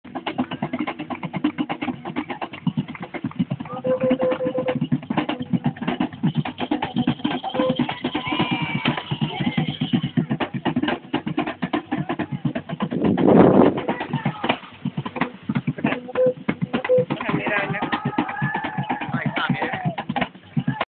video Garifona
este dio es interesante, este lo tome cuando yo estaba con los garifonas
Fíjense en la cosa que paso sobre el fuego parece una paloma pero no lo es
y despues desaparece.

Walter monge